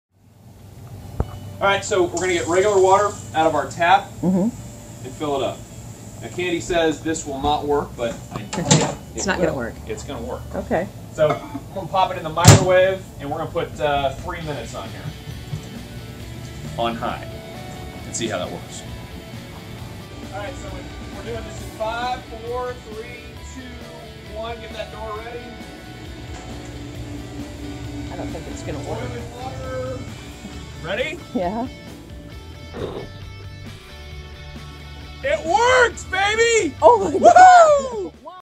How to Flash Freeze Boiling Water and make it snow
Even in Pensacola it can get really cold.  To make Boiling Water Flash Freeze you need to be careful and temperatures need to be in the 20's.  Start with a 3/4's full coffee cup of regular tap water. Put it in the Microwave for 3 minutes on High.  Then carefully take it outside where temps are in the 20's or lower and heave the water in the air away from you (hold on to the cup handle tight).   The water will separate in the air and flash freeze becoming a fine crystal ice mist.